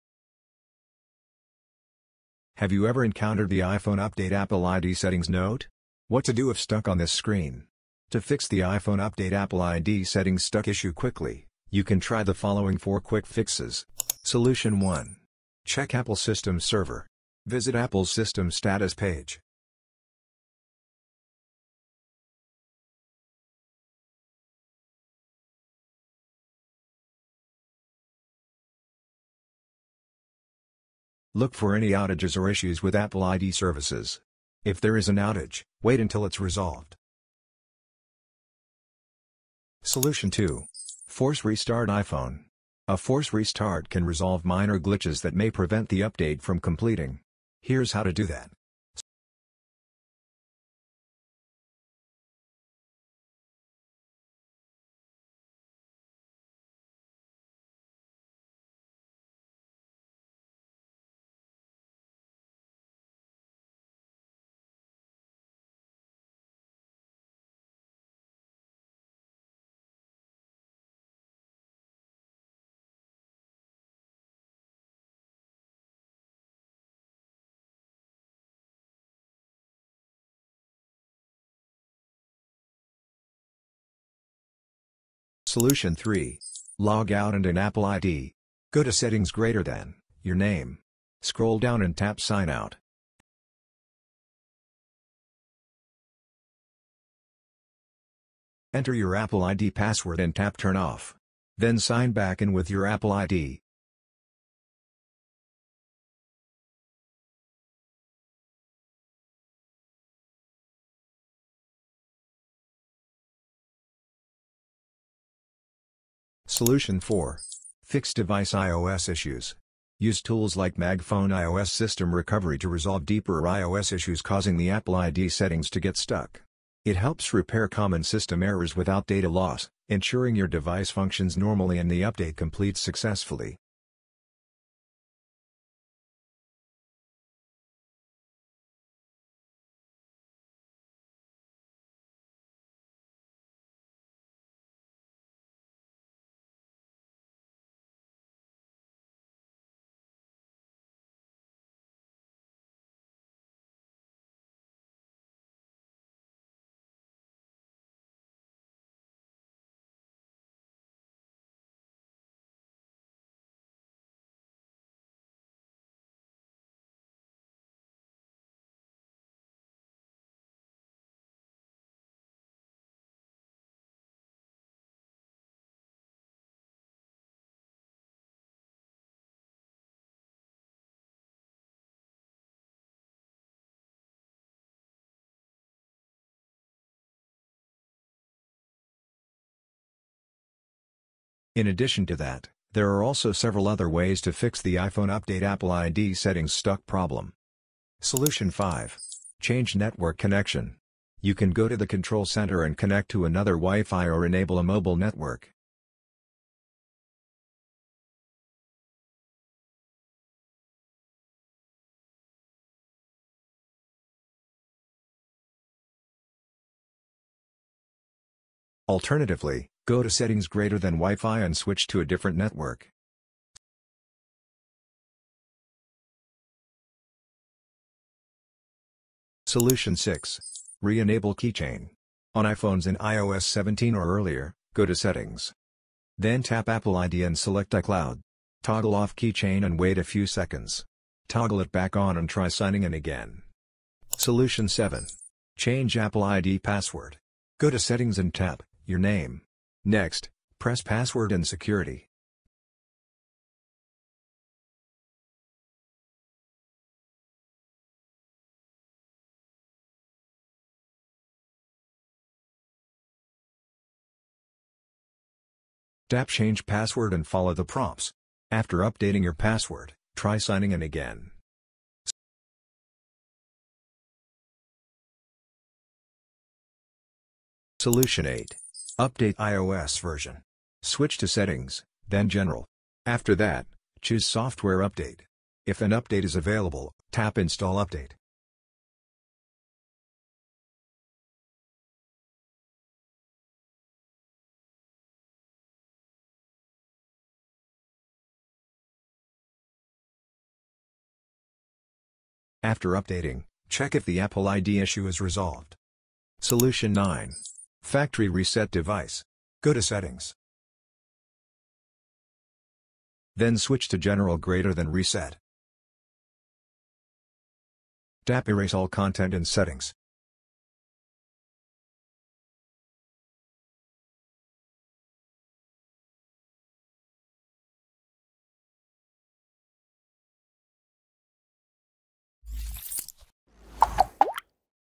iPhone Update Apple ID Settings Stuck? 9 Quick Fixes! [Latest]
If your iPhone is stuck on "Update Apple ID Settings," worry not. This video offers 9 quick fixes to resolve the issue. Whether it's an Apple ID authentication problem or a system glitch, these solutions will help you get your iPhone back to normal in no time. No need for complicated steps—just simple, effective fixes. Let’s dive in.
Use MagFone iOS System Recovery to fix the Update Apple ID Settings stuck issue

Method Covered:
0:00 Introduction
Four Quick Ways:
0:13 Solution 1. Check Apple System Server
0:39 Solution 2. Force Restart iPhone
1:25 Solution 3. Log Out and In Apple ID
1:56 Solution 4. Fix Device iOS Issues
Other Ways:
3:17 Solution 5. Change Network Connection
3:49 Solution 6. Reenable Keychain
4:03 Solution 7. Change Apple ID Password
4:37 Solution 8. Update iOS Version
5:06 Solution 9. Factory Reset Device